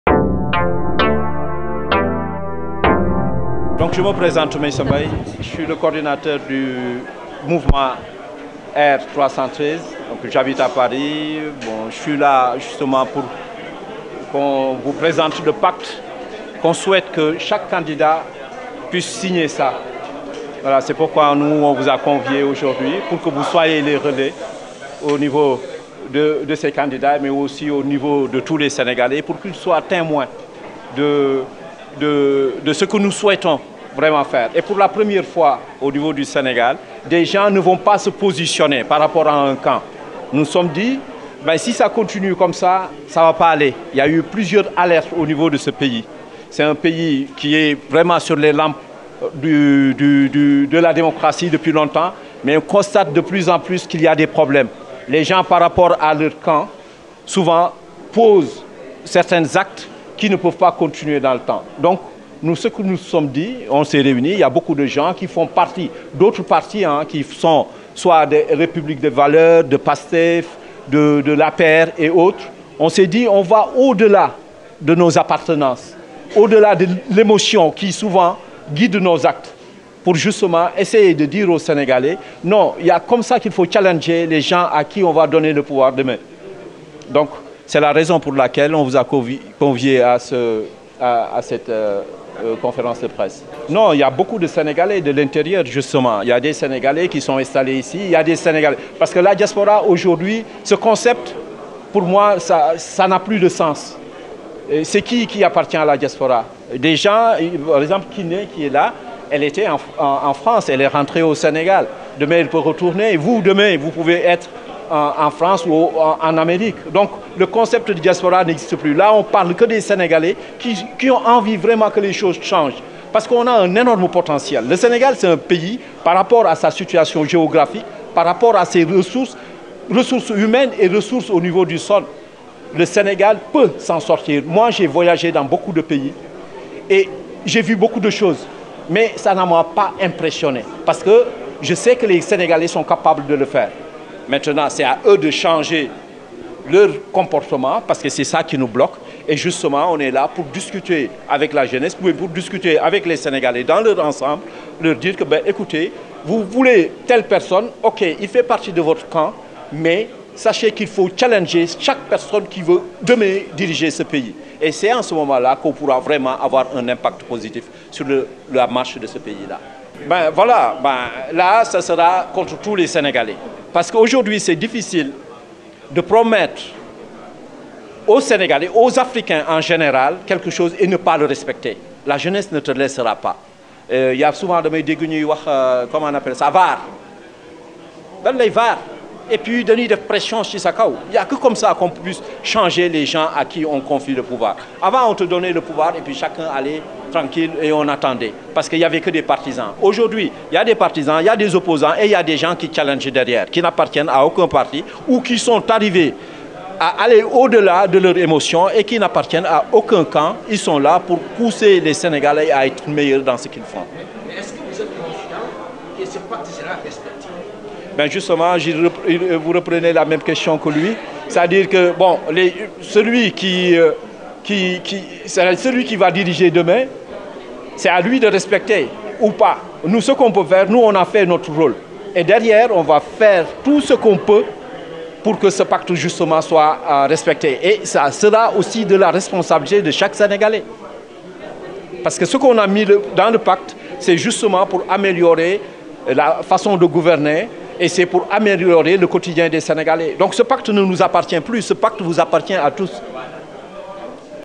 ELITE NEWS SN - Mouvement AIRS 313, cérémonie de lancement du pacte d'engagement
SOCIETE
Le mouvement AIRS 313 lance un pacte d'engagement pour le nouveau président de la république du Sénégal